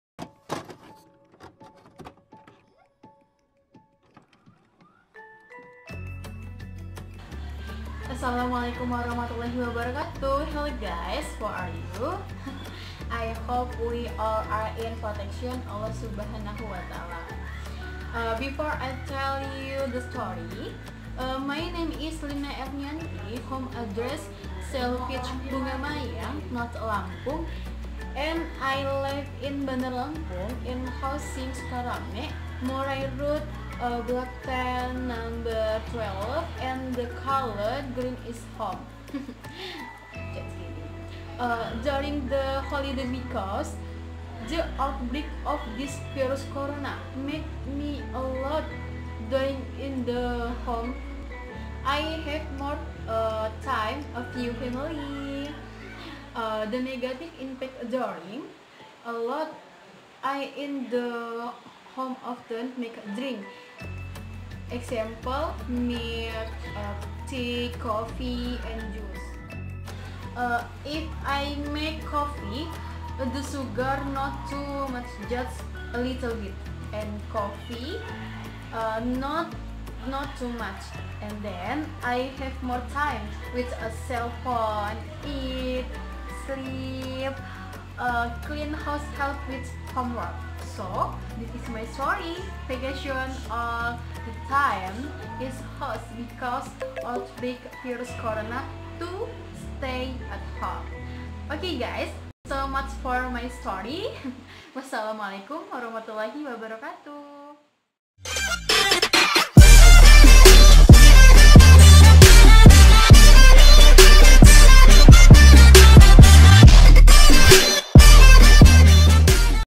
Stay at home because covid-19
Assalamualaikum,
Name : Lina erniyanti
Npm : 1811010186
Class : K/PAI/SMT4
Dosen : Meita Dwi Solviana, M.Pd.

Hello guys
I hope we all are in protection Allah swt.
Hhehehe
Before I tell you the story, my name is LINA ERNIYANTI
home address salvage bunga mayang, north lampung, and I life in bandar lampung, in housing, Sukarame, murai road, black ten, number twelve and the color green is home
Heheheheh
During the holidays becouse  the outbreak of this virus corona.
Make me a lot doing in the home
 I have more time a few family
The negative impact during a lot
I in the home often make drinks. Ex: milk, tea, coffe, and juice
If I make coffe, the sugar's not too much just little bit and coffes's not too much
 And then...
I have more time with a cell phone, eat, sleep, clean house help with homework
So,,, this is my story
Vacation all the time in home because, outbreak virus corona to stay at home
Oke guys
So much for my story Hehehehe
Wassalamualaikum wr.wb